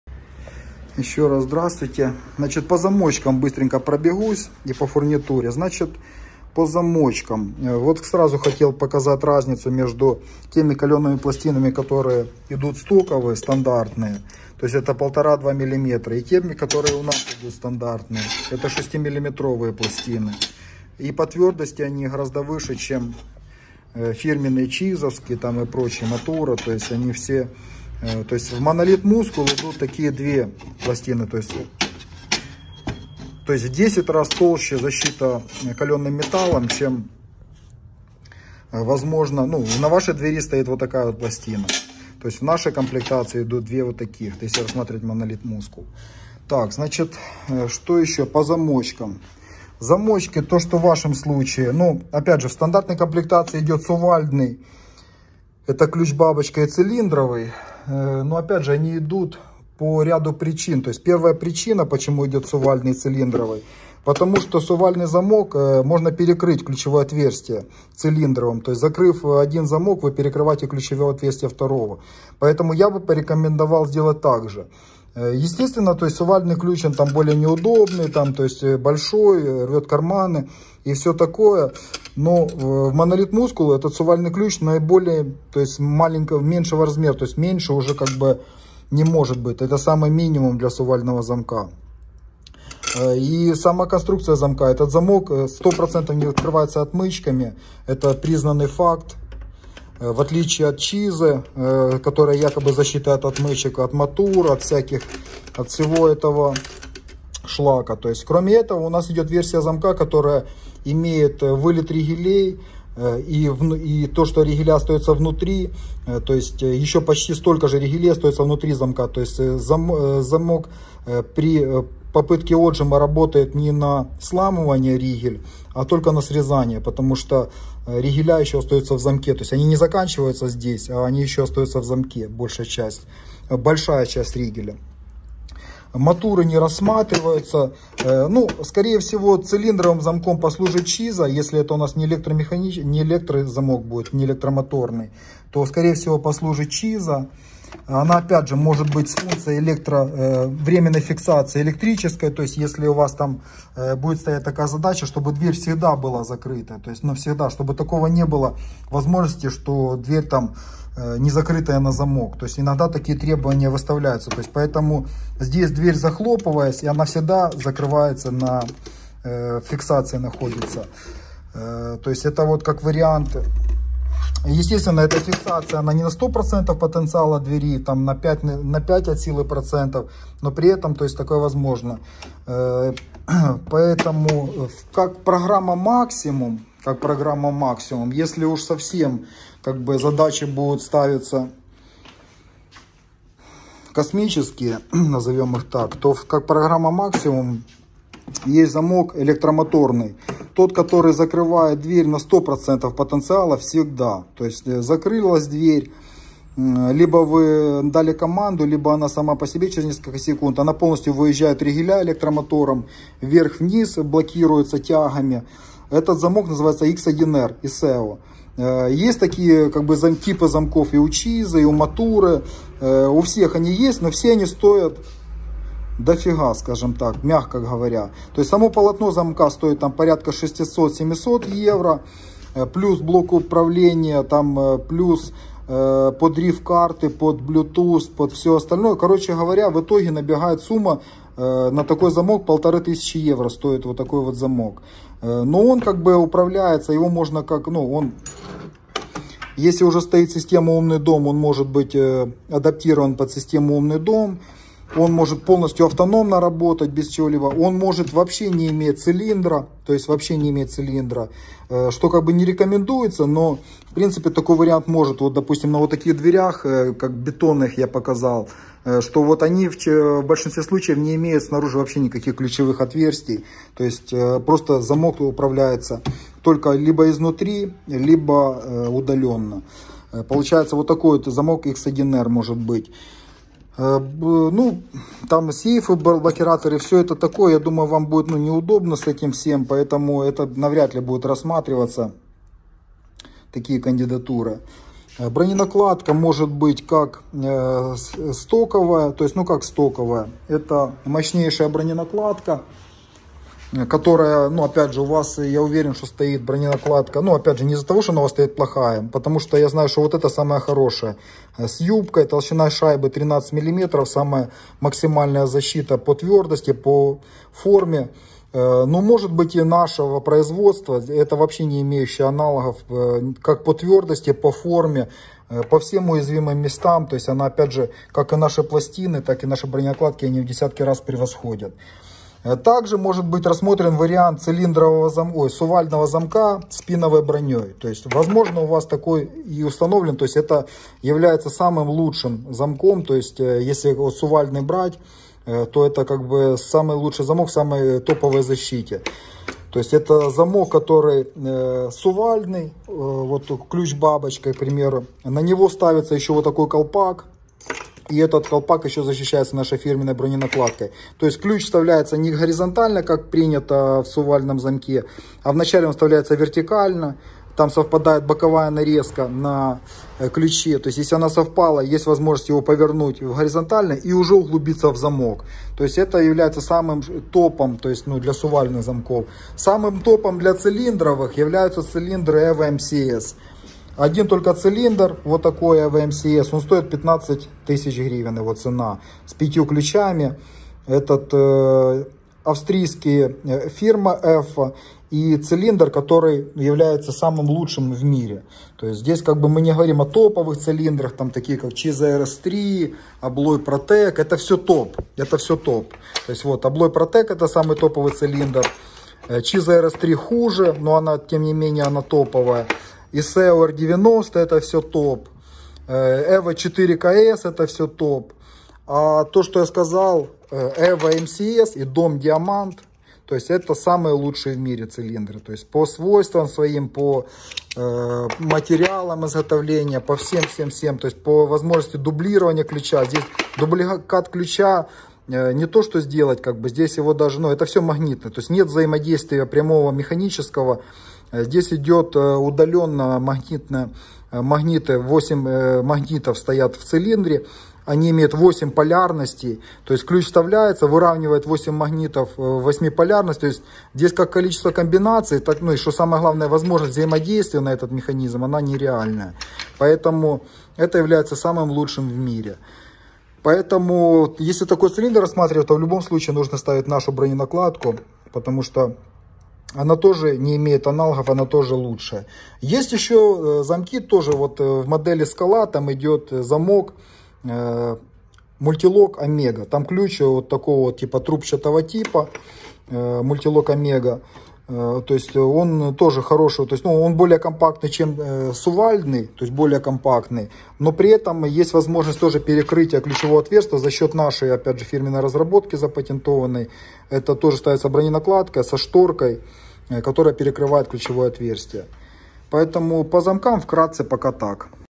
Дверные замки, врезные, цилиндровые, сувальдные и броненакладки. Лучшие комбинации для дверей.
В этом видео мы консультируем заказчика, у которого собралось несколько вопросов по замковым группам. Пройдемся по самым популярным дверным замкам, их плюсам и минусам.
Цилиндровый замок и сувальдный замок в одной двери дают хорошую взломостойкую комбинацию. В тандеме с нашей фирменной перезапорной системой, хорошо подобранные дверные замки имеют полную защиту от отмычек.

ООО "Монолит - двери и сейфы"
Производство бронированных дверей и взломостойких сейфов.
Обращайтесь!
г. Киев, ул. Вискозная, 3Б